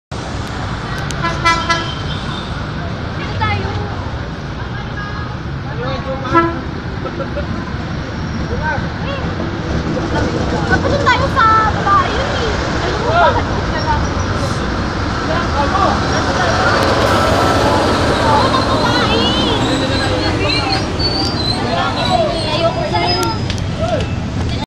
PNR DMU set 3 arriving at Vito Cruz Station
PNR DMU set 3 performing T-1507 arriving at Vito Cruz Station. :)